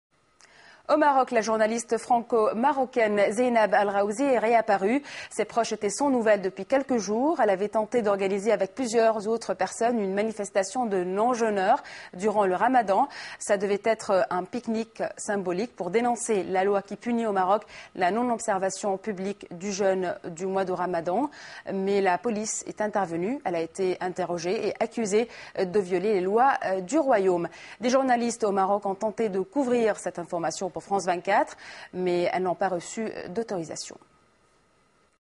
Les Athées au Maroc
les Athées au Maroc rompent le jeûne en plain Ramaden